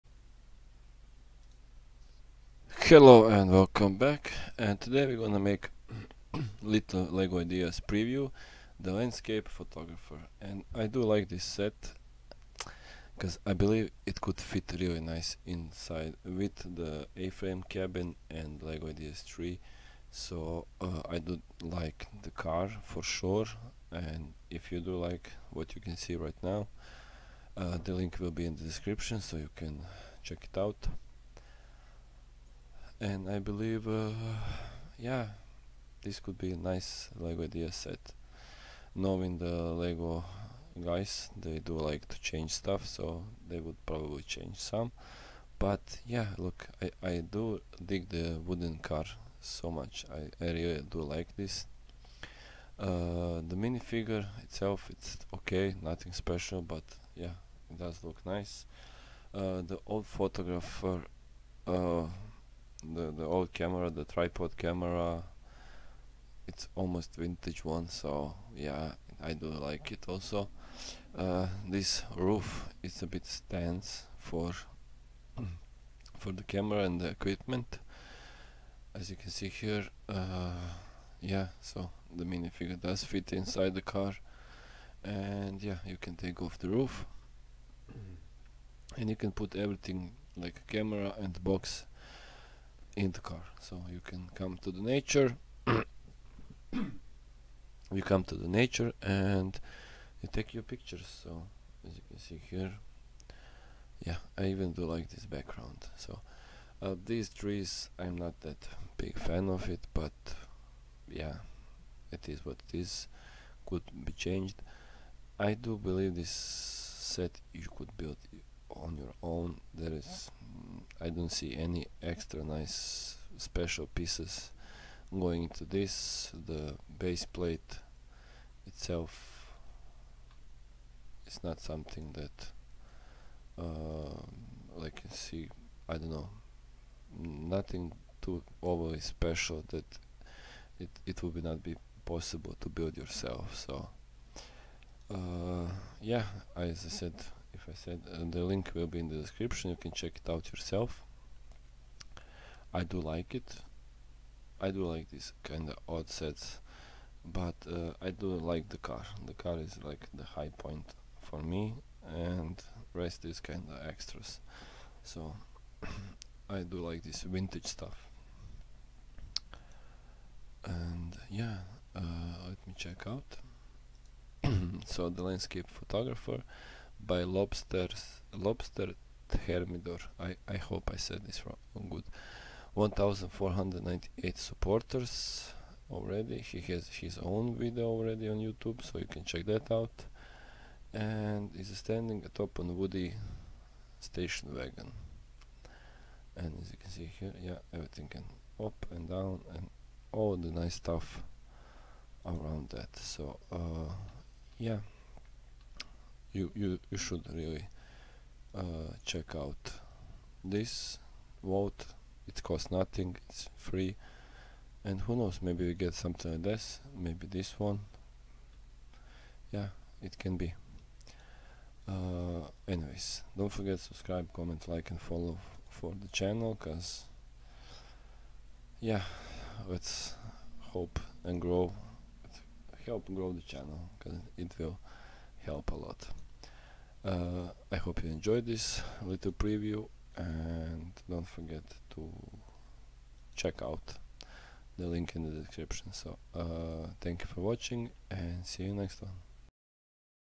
LEGO IDEAS THE LANDSCAPE PHOTOGRAPHER By LobsterThermidor
The photographer is standing atop a woody station wagon reminiscent of one used by Ansel Adams in a similar fashion in order to achieve the best view for the composition.
The car’s wooden panels, as with the dirt road upon which it is parked, are examples of the ways in which humans have merged nature with invention.